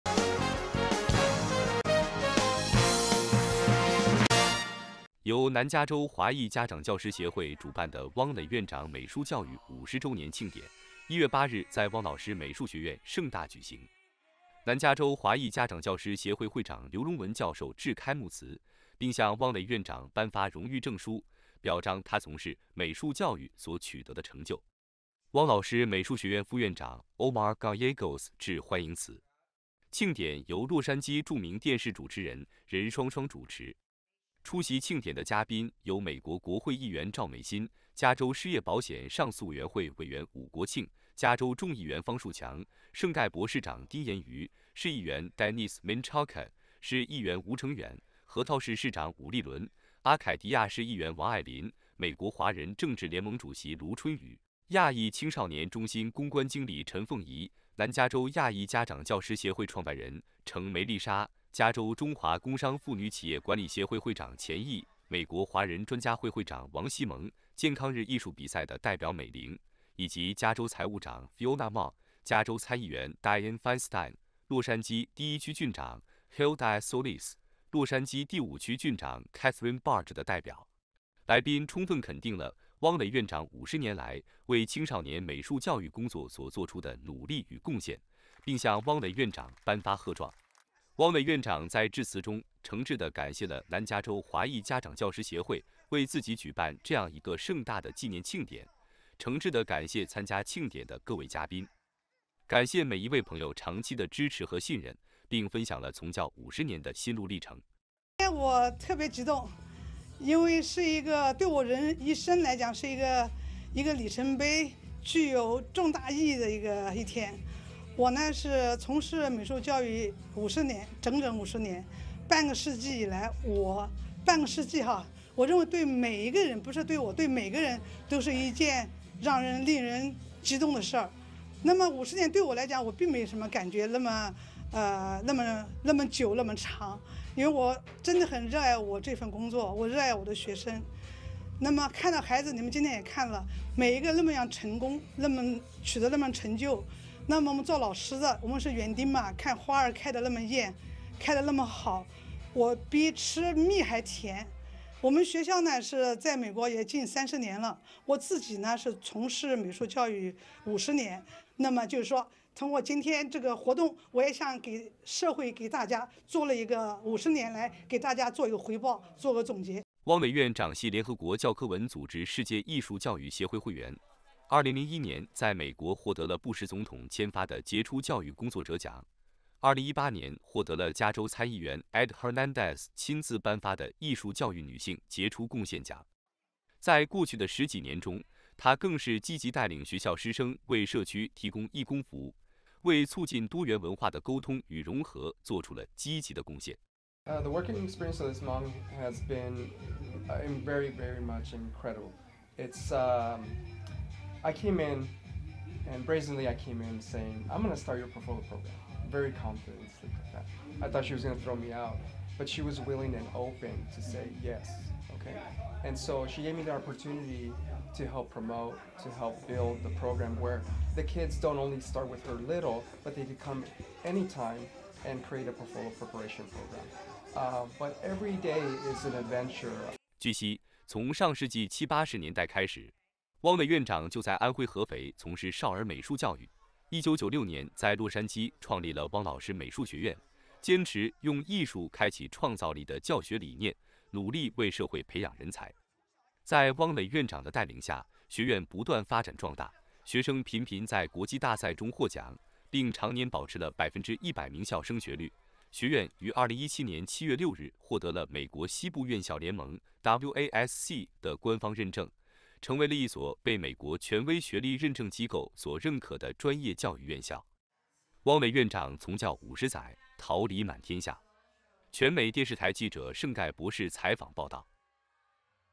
從教五十載，桃李滿天下 汪蕾院長美術教育五十周年慶典1月8日盛大舉行【AMTV】
由南加州华裔家长教师协会主办的“汪蕾院长从事青少年美术教育50周年庆典（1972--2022）”于2023年1月8日下午在汪老师美术学院盛大举行。庆典由南加州华裔家长教师协会会长刘荣文教授致开幕辞，加州现任民主党参议员Dianne Feinstein代表、美国国会议员赵美心、加州失业保险上诉委员会委员伍国庆、美国加州众议院议员方树强、白冬梅代表美国华裔民选官员协会创办人黄赵企晨（加州财务长Fiona Ma贺状）、洛杉矶第一区郡长Hilda Solis代表、洛杉矶第五区郡长Kathryn Barger代表、圣盖博市长丁言愉、圣盖博市议员Denise Menchaca、圣盖博市议员吴程远、核桃市市长伍立伦博士、阿凯迪亚市议员王爱琳、美国华人政治联盟主席卢春雨、亚裔青少年中心公关经理陈凤仪、南加州亚裔家长教师协会创办人程梅丽莎、加州中华工商妇女企业管理协会会长钱㦤、美国华人专家会会长王希萌、健康日艺术比赛代表人美伶等人出席庆典，分别致辞并向汪蕾院长颁发了贺状。

国会议员赵美心在致辞中高度评价了汪蕾院长长期致力于青少年美术教育的杰出贡献，充分肯定了汪蕾院长为社区带来的长远而积极的影响，并祝愿她在今后的工作中取得更大的成就。

南加州华裔家长教师协会会长刘荣文教授致词，高度评价汪蕾院长半个世纪以来一直从事青少年美术教育，热心公益事业，教书育人，桃李满园，成为教师的楷模。因此，他代表南加州华裔家长教师协会向汪蕾院长颁发荣誉证书。

美国华人政治联盟部分理事向汪蕾院长表示祝贺，圣盖博市议员吴程远，政治联盟副主席沈丽玉，华权会主席、政治联盟副主席江志高，政治联盟副主席黄正芬，华权会副会长、圣盖博狮子会副主席、美国大西北总商会执行主席贺晓慧，亚凯迪亚市议员王爱琳，汪蕾院长，政治联盟主席卢春雨博士，圣盖博市长、政治联盟名誉主席、圣盖博狮子会会长丁言愉，政治联盟理事孔建英

汪蕾院长在致辞中诚挚地感谢了南加州华裔家长教师协会为自己举办这样一个盛大的纪念庆典，诚挚地感谢参加庆典的各位嘉宾，感谢每一位朋友长期的支持和信任：“今天，对于我的一生来说，都是一个具有里程碑意义的、非常重要的日子。此刻我站在这里，心情是极为激动的。从事青少年美术教育整整50周年，对于这长达半个世纪的一个总结、回顾和纪念，对任何人来说，意义都是非同小可的 。”

从上世纪七八十年代开始，汪蕾院长就在安徽合肥从事少儿美术教育，1996年在洛杉矶创立了汪老师美术学院，坚持“用艺术开启创造力”的教学理念，努力为社会培养人才。在汪蕾院长的带领下，学院不断发展壮大，学生频频在国际大赛中获奖，并常年保持了100%名校升学率，学院于2017年7月6日获得了美国西部院校联盟(WASC)的官方认证，成为了一所被美国权威学历认证机构所认可的专业教育院校。

“回顾50年来自己的成长足迹，我为自己能够切实地为教育下一代、传播艺术文化尽一份绵薄之力而感到欣慰。我在不少场合也说过，这辈子我只对一件事感兴趣并且真的做到了几十年如一日的专注，那就是教孩子们画画。”汪蕾院长说，“从我们学校毕业的学生，他们有的被艺术名校录取，包括Art Center, Otis, RISD, Parsons, SVA等顶尖艺术类大学，还有很多同学申请的都是他们最心仪的综合类大学，历年来我们的学生被包括哈佛，耶鲁，普林斯顿，哥大综合类名校录取，我们培养的不仅仅是未来的艺术家，更是各行各业具有创新精神的领导型人才。”

汪蕾院长系联合国教科文组织世界艺术教育协会会员，2001年在美国获得了布什总统签发的“杰出教育工作者”奖，2018年获得了加州参议员Ed Hernandez亲自颁发的“艺术教育女性杰出贡献”奖。在过去的十几年中，她更是积极带领学校师生为社区提供义工服务，参与主办、协办与艺术相关的社区活动达百余次，义工服务时间超过5,000小时，包括：组织美术比赛，协助相关教育机构筹款，组织为国际贫困儿童募捐等等，为促进多元文化的沟通与融合做出了积极的贡献。

庆典上还特邀到场官员与嘉宾为学院的学生们颁发了奖杯与奖状。在过去的三年中，学生们参加各项美术比赛获得了不少奖项，但因疫情关系很多颁奖典礼都取消了。借此盛典之机由嘉宾们为学生们补颁奖杯和奖状，无疑对学生们会起到极大的鼓舞作用。